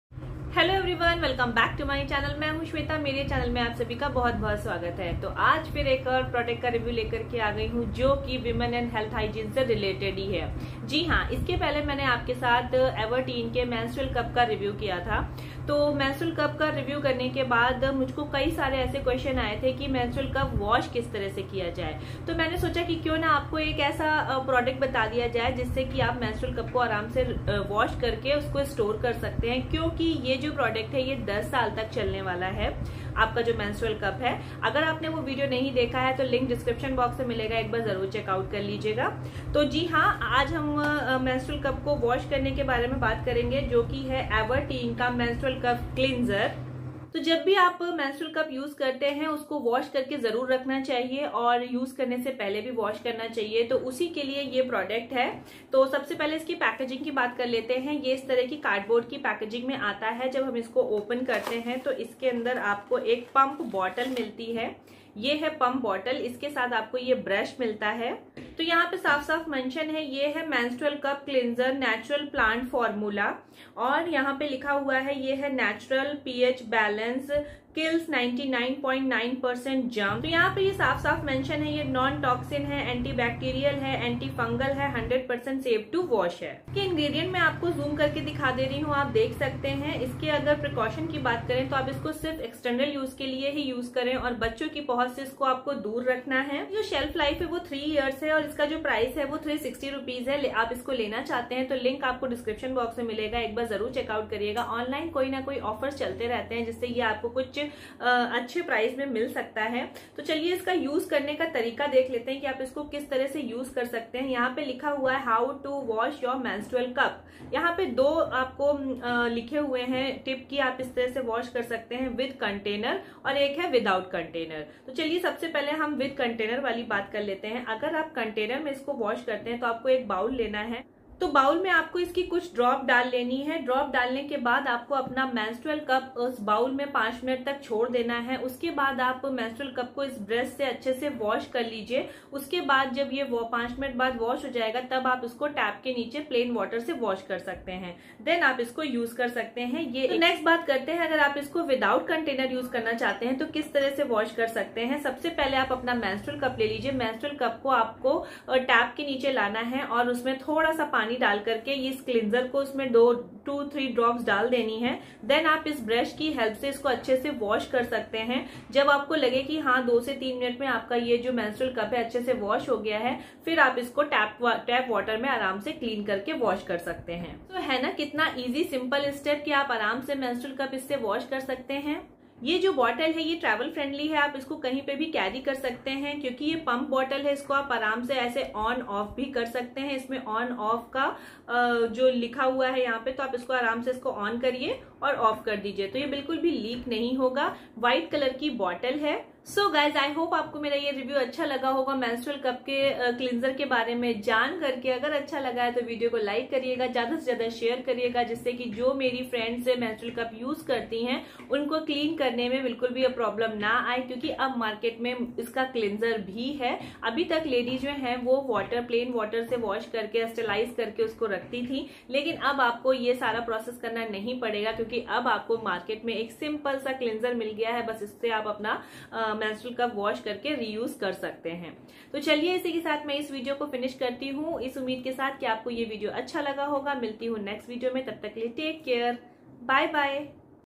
The Right to way to clean Menstrual Cup Cleanser Review in Hindi/indianvloggerbulbul
Stop visiting the kitchen to wash your menstrual cup.

Get your Menstrual cup clean in just 5 mins with everteen Menstrual Cup Cleanser. It has:

Antibacterial properties
100% natural actives
Made up of natural plant extracts